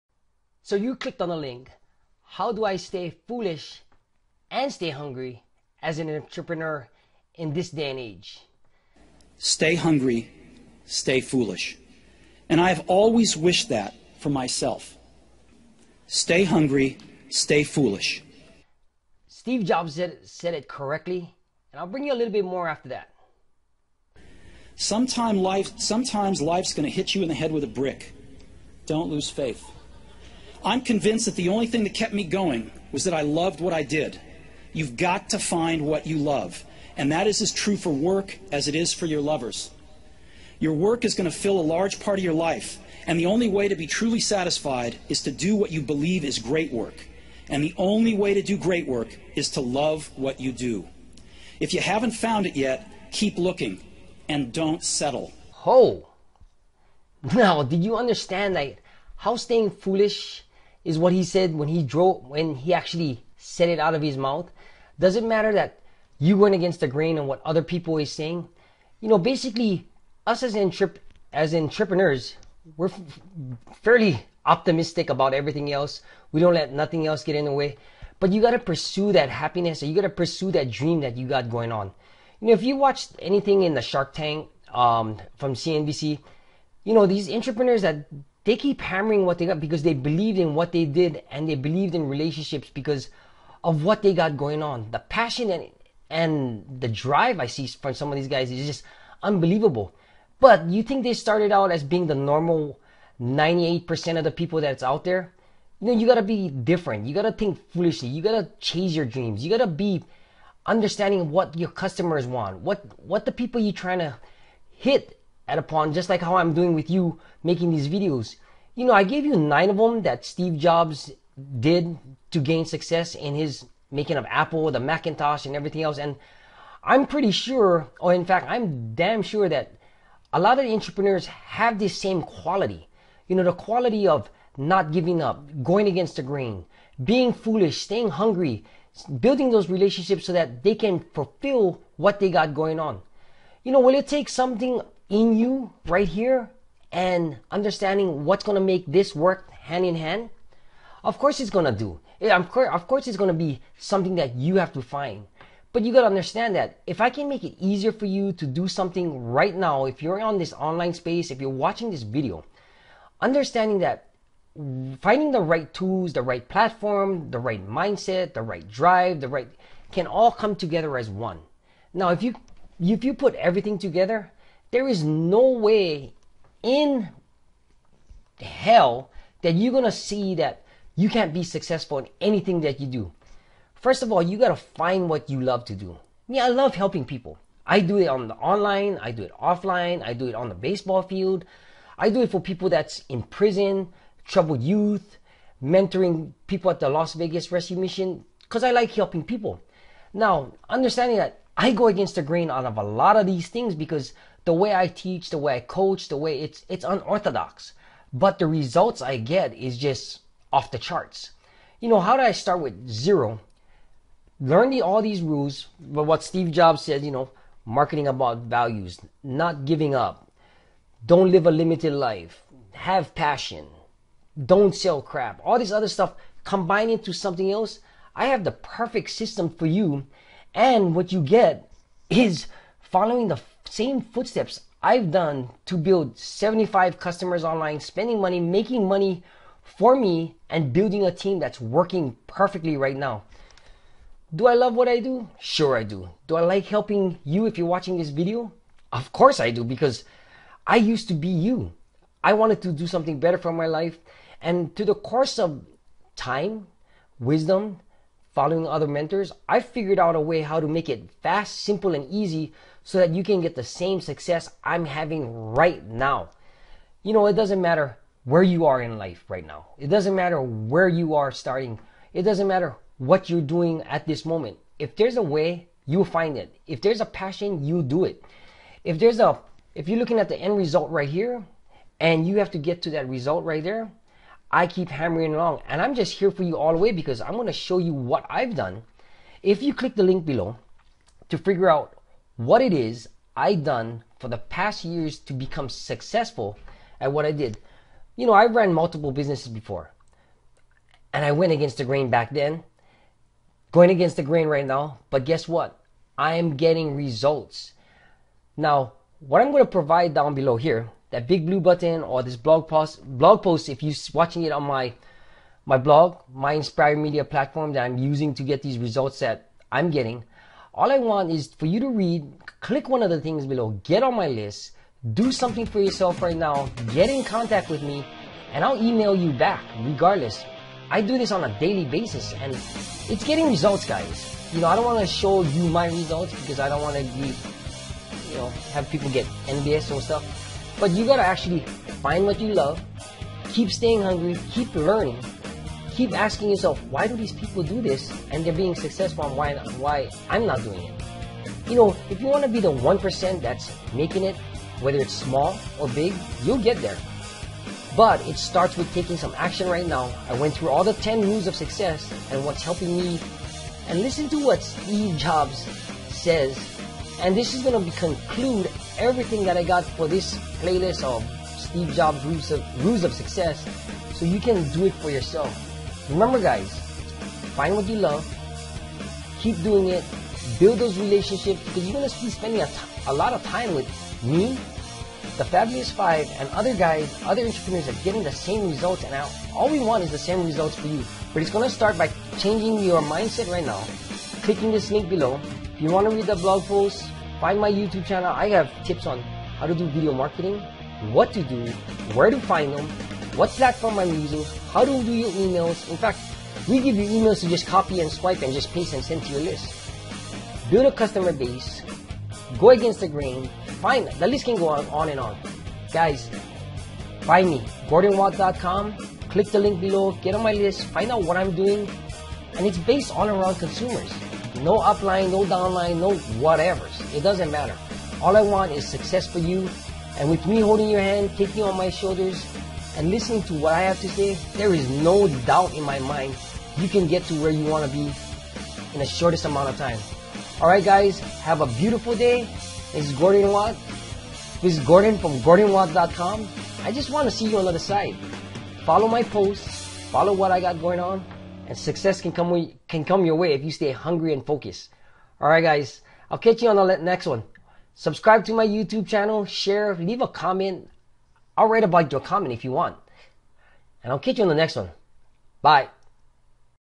How Do I Stay Foolish and Hungry as an Entrepreneur
Staying foolish and Hungry as a business person entrepreneur is one of the biggest attributes anyone can have. Now looking for that spark is what sets us apart from the rest of the 98% that are out there.

Are you a person that likes to help others, do things for the right reason, or just want something more for you and your family? I might be able to team up with you if you are one that I want to work with.